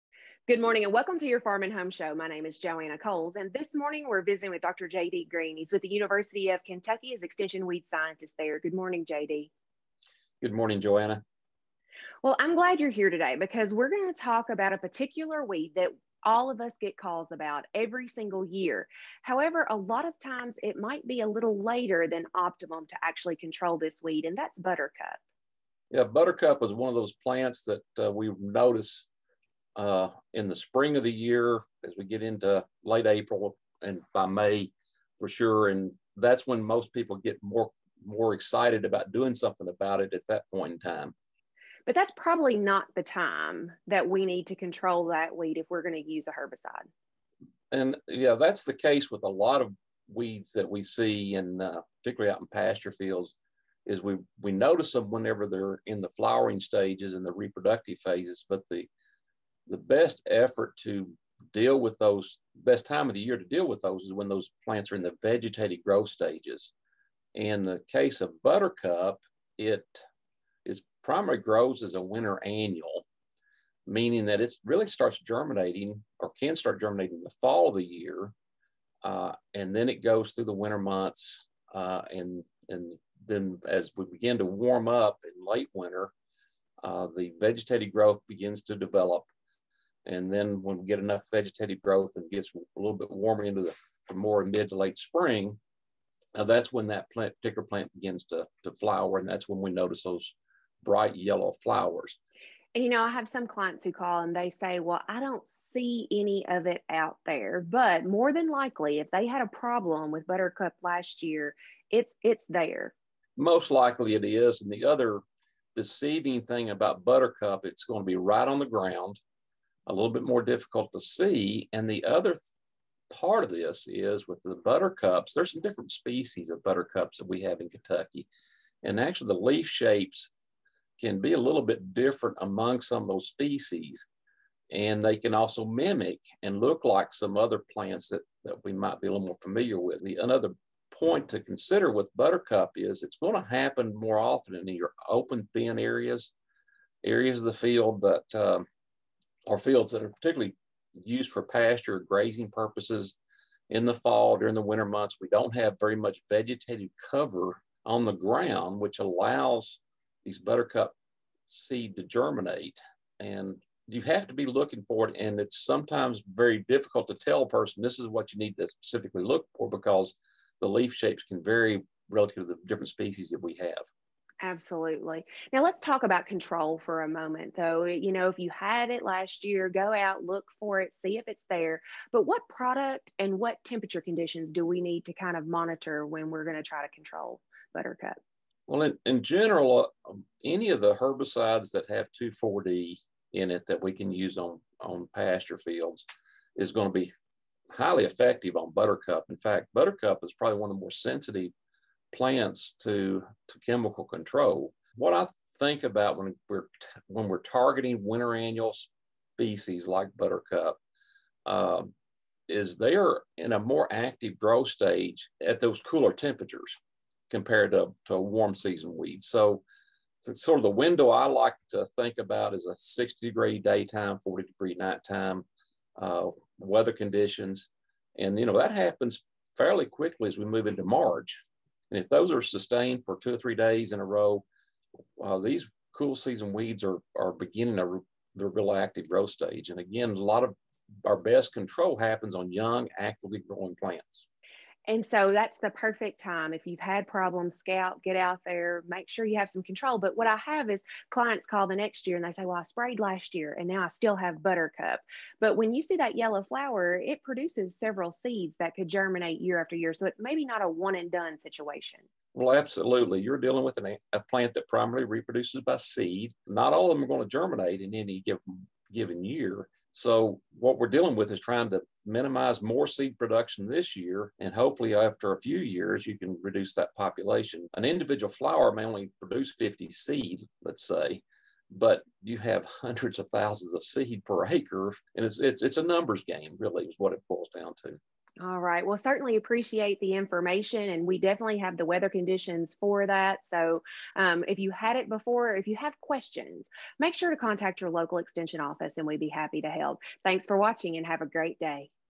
Yellow Buttercup Weed Control
Joanna visits with Dr. JD Green, Extension Weed Scientist to discuss Yellow Buttercup Weed Control.
One of the best times of the year to assess fields and fencerows for presence of cool-season weeds is in late winter. Furthermore, the preferred time to implement control tactics can often be in March as daytime air temperatures begin to rise and are maintained above 55F. This is when cool-season weeds are younger and begin their active vegetative growth before initiating flowers later in the spring. There are various species of buttercup (Ranunculus spp.) with Bulbous, Creeping, Hispid, Tall, and Smallflower buttercup being some of the more likely species found in Kentucky. Buttercup is more frequently found in fields or field areas that are utilized or heavily grazed in the fall and throughout the winter months. This results in thin, bare areas throughout the field with little vegetative cover creating an environment whereby buttercup seed can readily germinate and seedling plants thrive. Therefore, one long-term control strategy involves utilizing management practices which help promote growth of desirable forage species and minimize bare areas. Interseeding more desirable forage species may be another practice to consider. This is not always practical in some fields that are essential for winter feeding areas. Another control option is herbicide treatment in early spring. Herbicide products that contain 2,4-D, or other broadleaf type pasture herbicides, are generally effective on most buttercup species. To be most effective herbicide treatment should be completed when plants are in the vegetative stages of growth before flowers develop and produce new seed.  Hence, herbicide applications should normally occur by late March. Treatments after flowering offer little benefit since buttercup plants are already producing new seed and plants die back naturally by late spring and will not be present the remainder of the year.